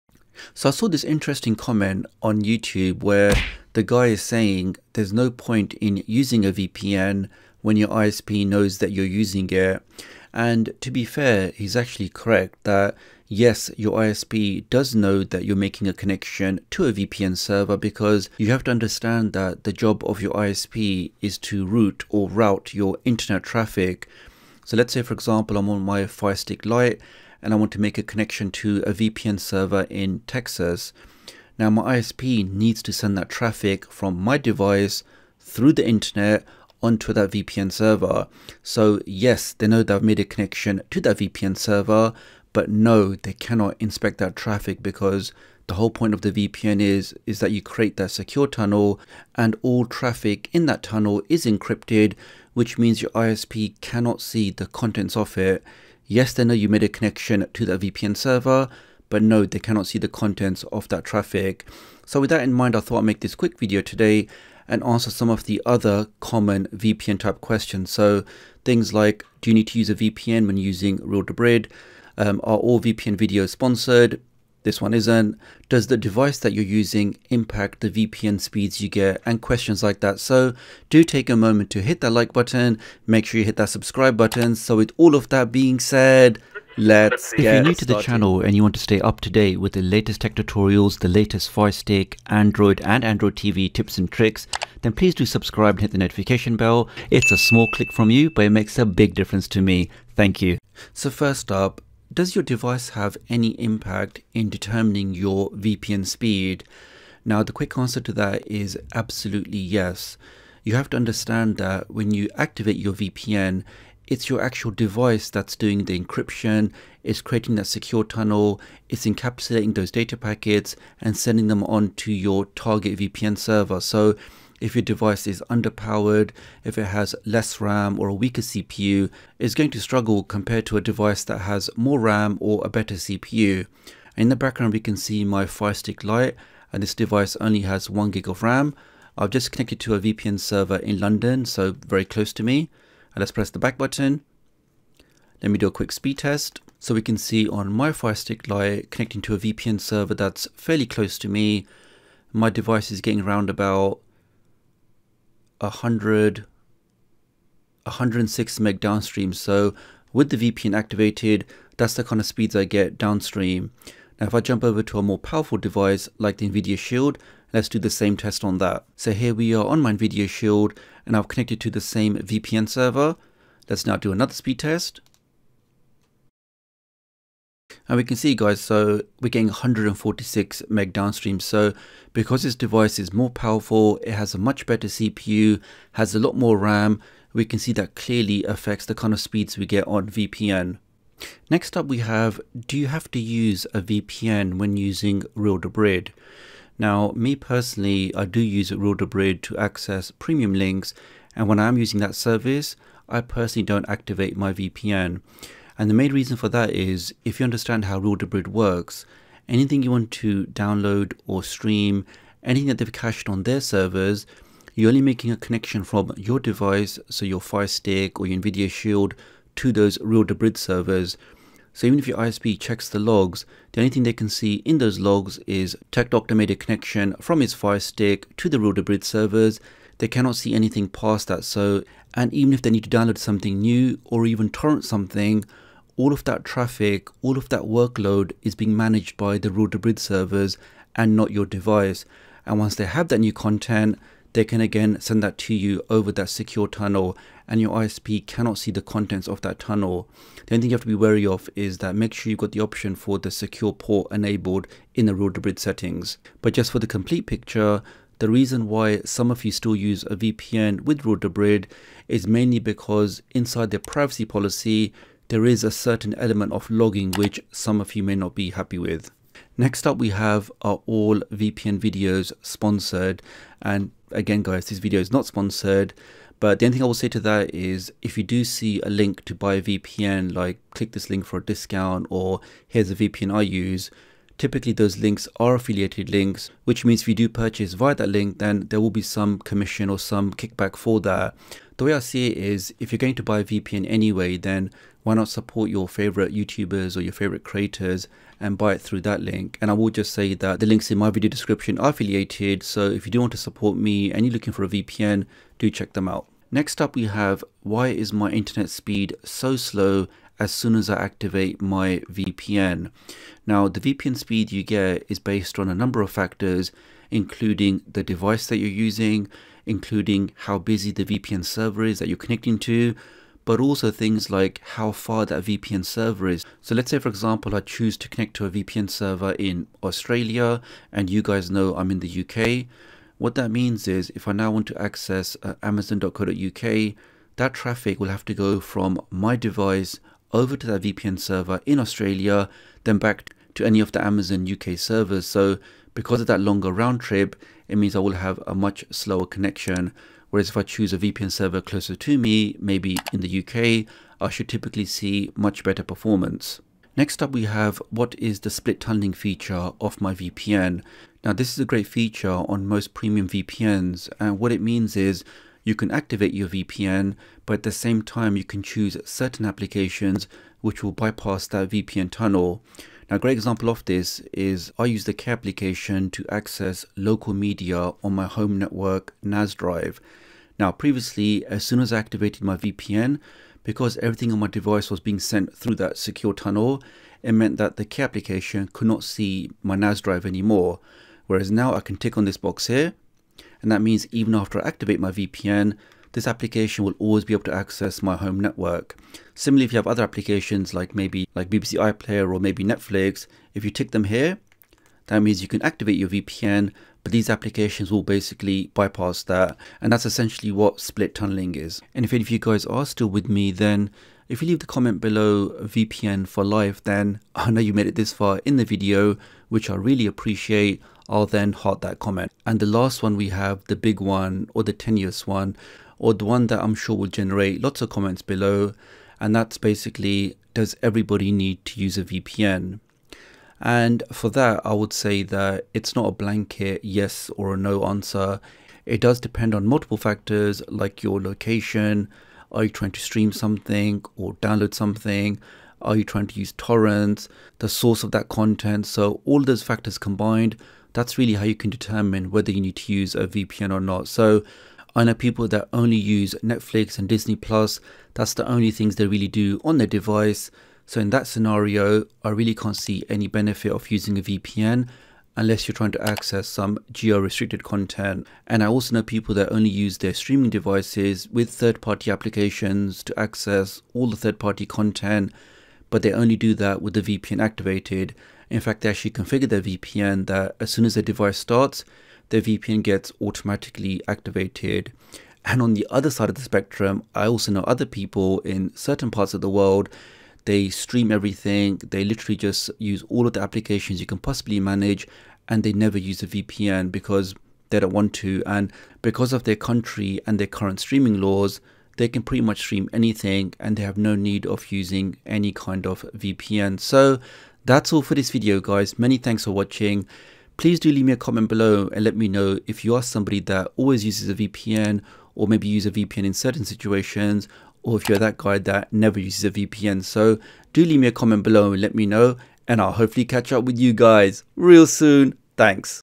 🔴YOUR ISP KNOWS YOU ARE USING A VPN (CONFIRMED !)
Wondering if your ISP does know that you have made a VPN connection. Do you want to know why your VPN speed is so slow? In this video I will answer the common VPN questions.